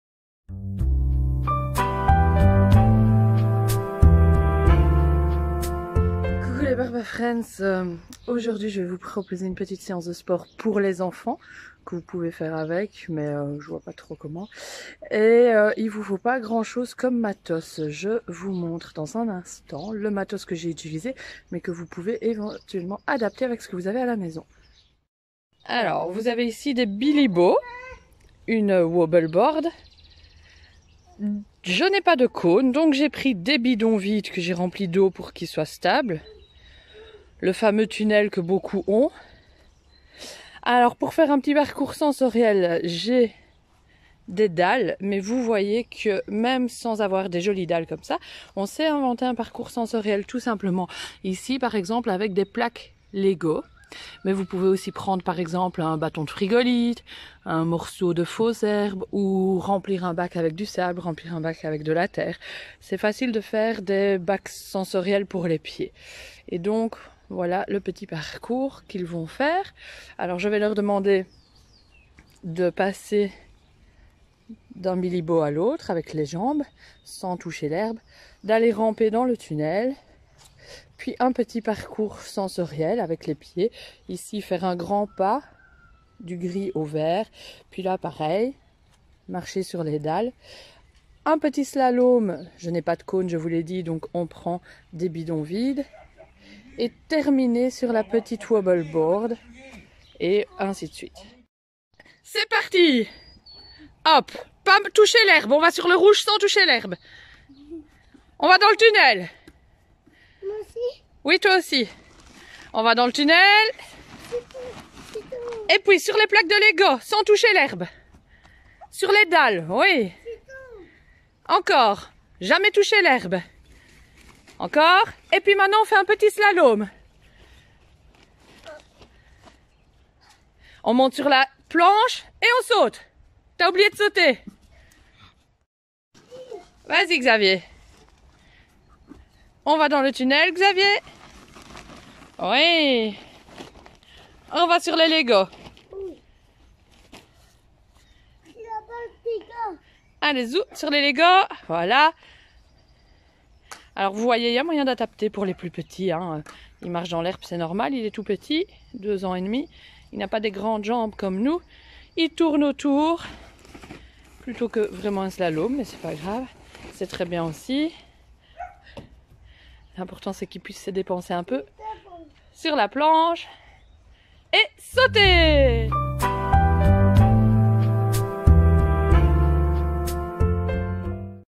gym extérieur kids
Petit parcours de gym, parcours sensoriel en extérieur pour occuper les jeunes enfants. Pas besoin de trop de matos.
Des bidons vides ou bouteilles vides, des plaques de légo, frigolite, un petit marche pied etc...Et le tour est joué.